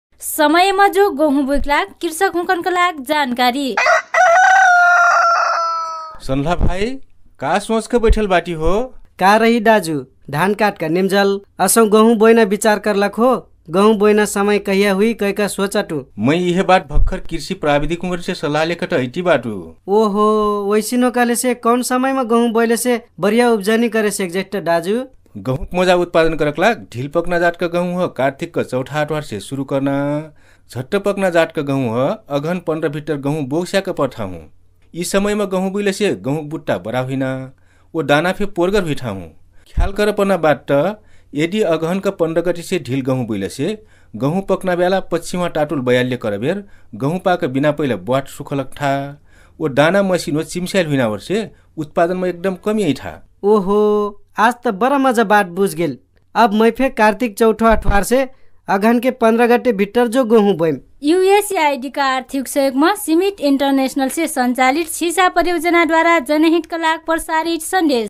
Promoting early sowing of wheat in Nepal (Tharu)
In Nepal, CSISA conducted a mass advertising campaign to encourage the early sowing of wheat in the FtF zone and in areas across the Terai where wheat is grown.  The jingles were produced in Nepali and in Tharu languages to reach farmers with Nepali and non-Nepali mother tongues. These dramas were broadcast by FM stations across the four Mid-West and Far Western Terai districts in October and November 2019 just before the wheat sowing time.